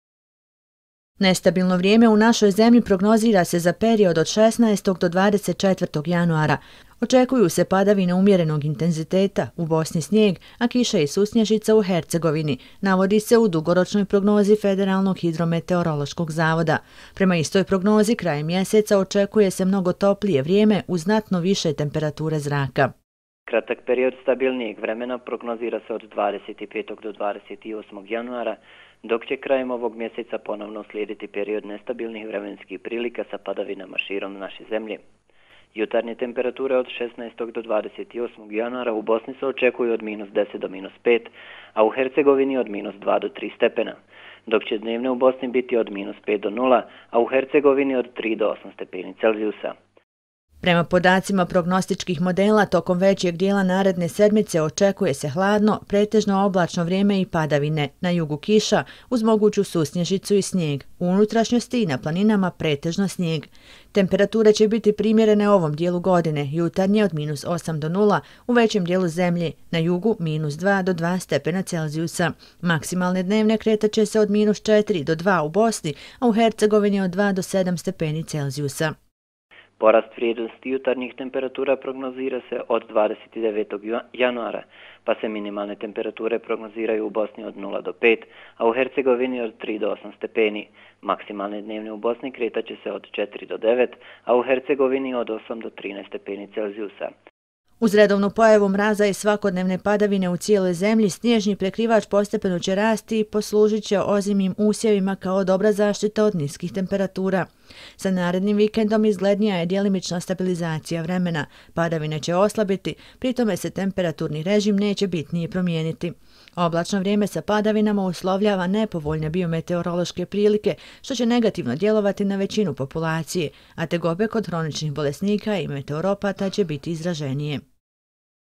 Januar ćemo ispratiti sa snijegom i temperaturama ispod nule - 17.01.2017.
Padavine umjerenog intenziteta praćene temperaturom ispod nule u našoj zemlji zadržat će se do početka naredne sedmice, tačnije do 25. januara od kada se očekuje nešto stabilnije vrijeme sa višim temperaturama. Stabilno vrijeme neće se dugo zadržati jer su već za kraj  mjeseca najavljene obilnije padavine i postepeni pad temperature.